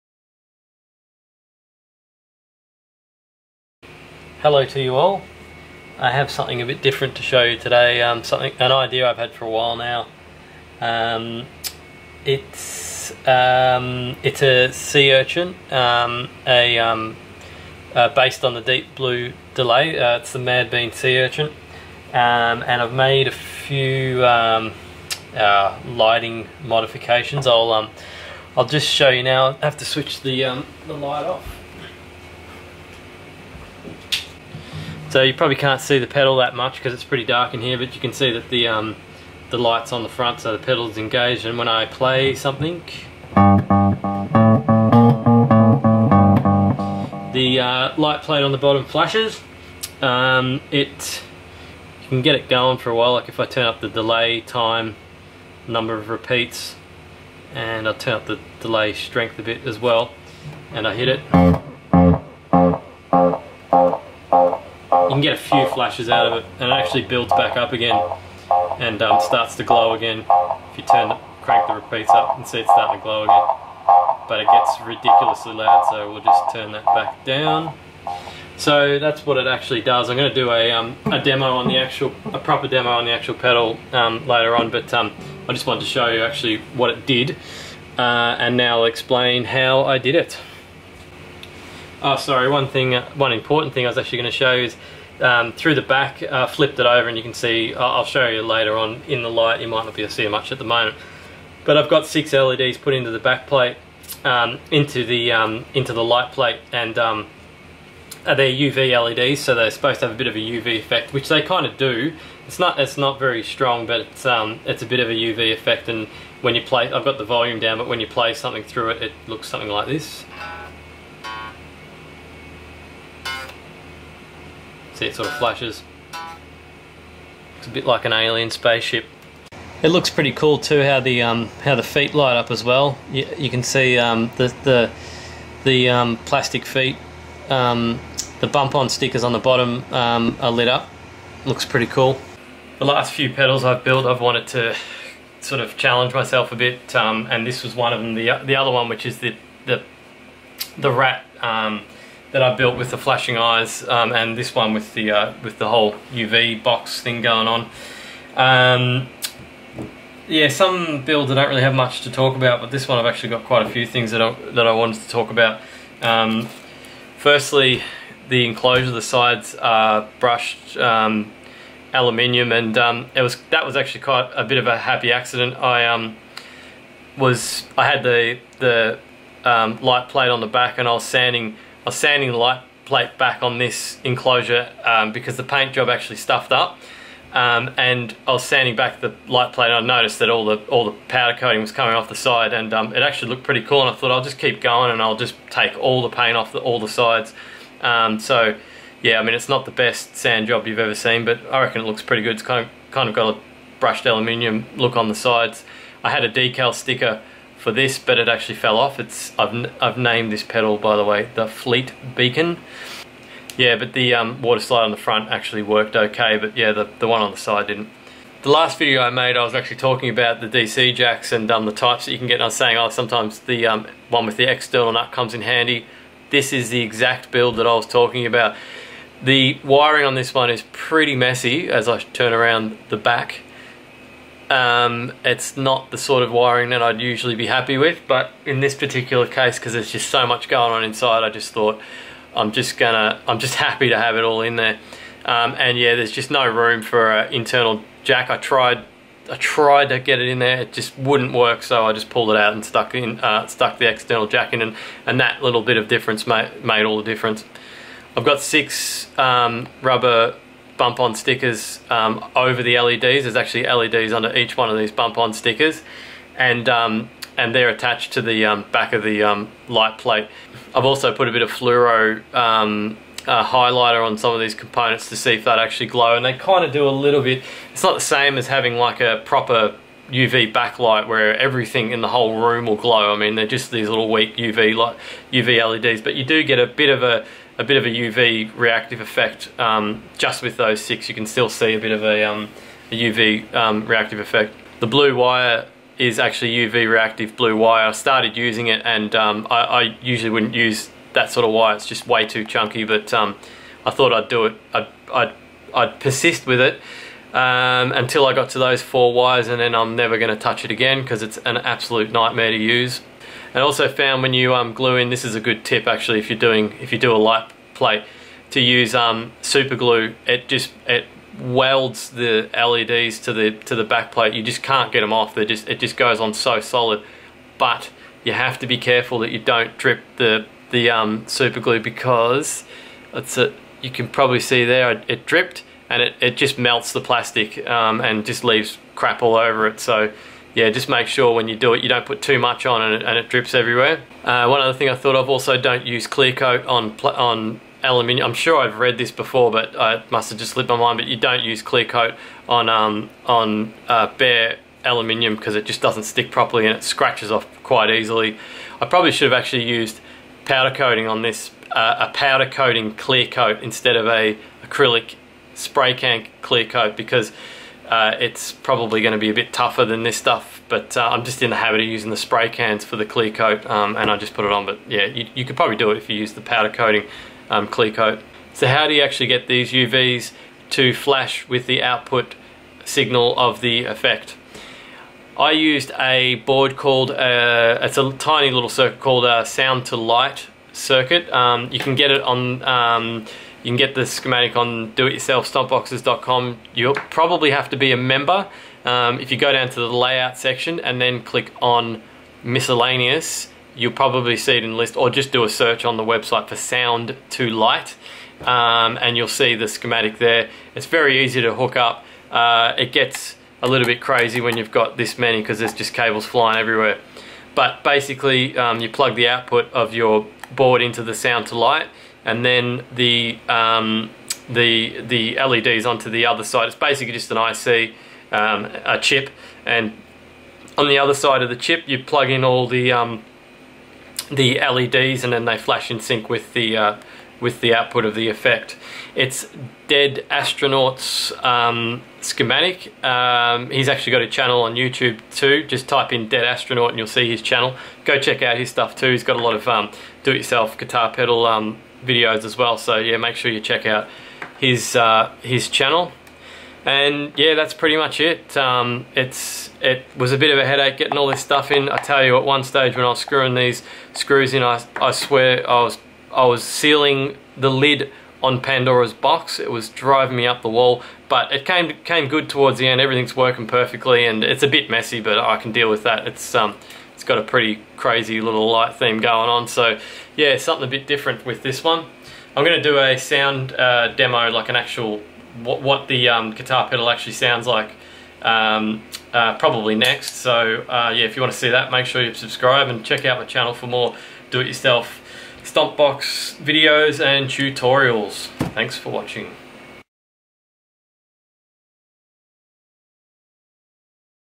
Build Report: Fleet Beacon Delay Based On Deep Blue Delay (Madbean Sea Urchin)
Build Report for the Fleet Beacon Delay based on the Deep Blue Delay (Madbean Sea Urchin).  Explanation of a uv light plate with Sound To Light circuit powering the LED's